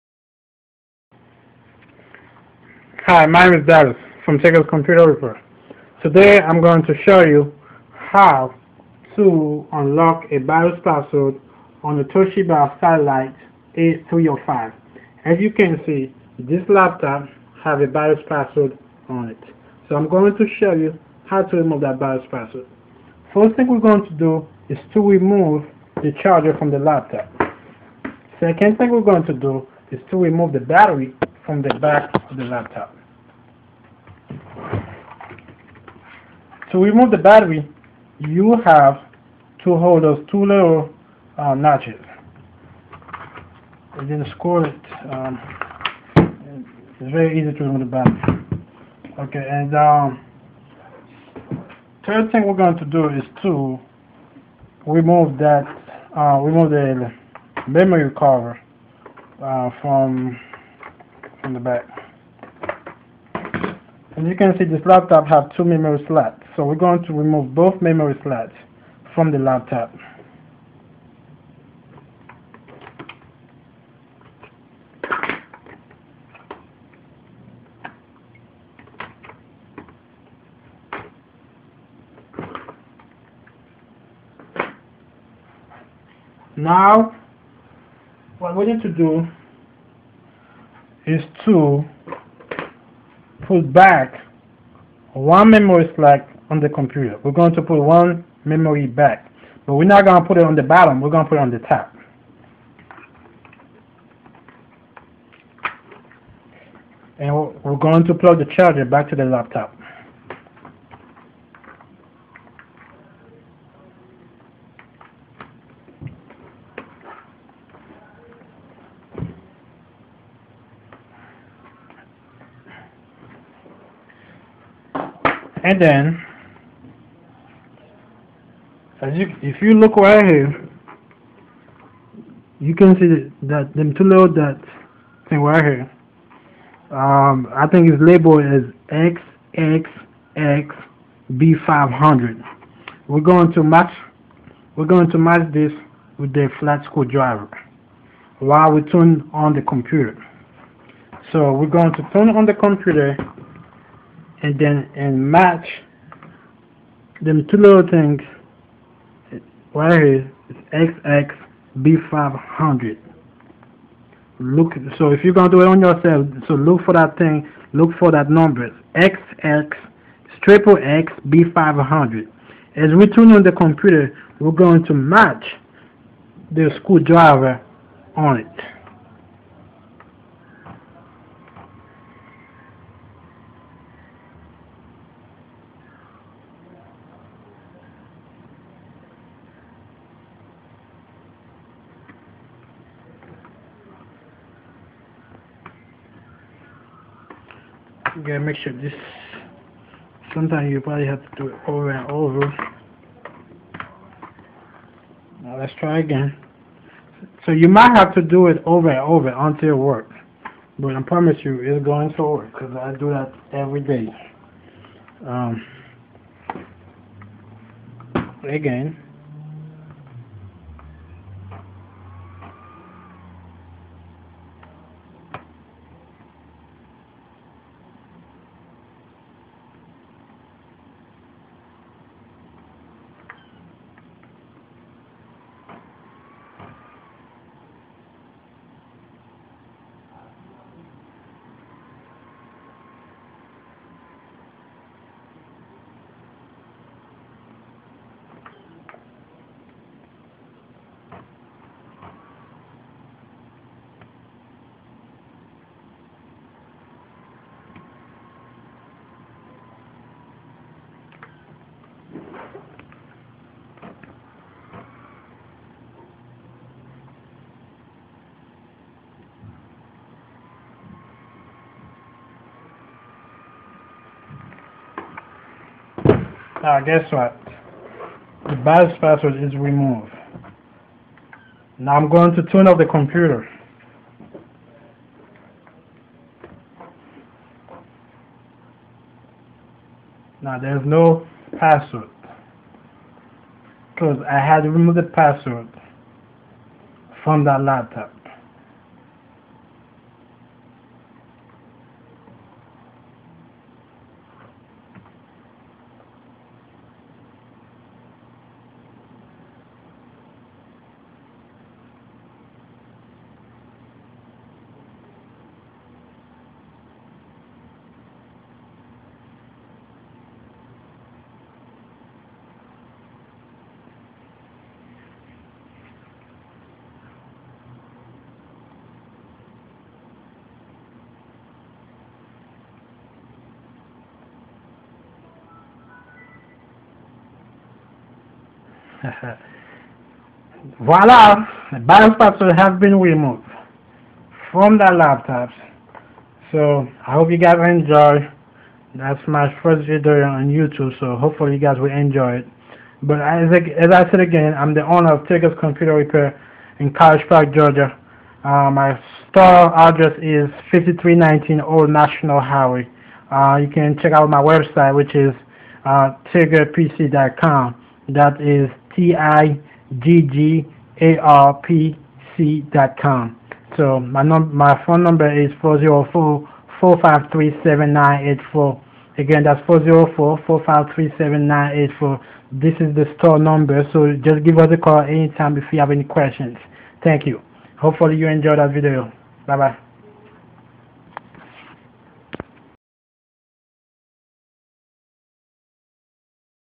HOW TO REMOVE BIOS PASSWORD ON A TOSHIBA SATELLITE A305 AND C655D
If you forgot your laptop  bios password we can unlock it for you go to

If the screw driver don't work try a wire cable curve it to a "U"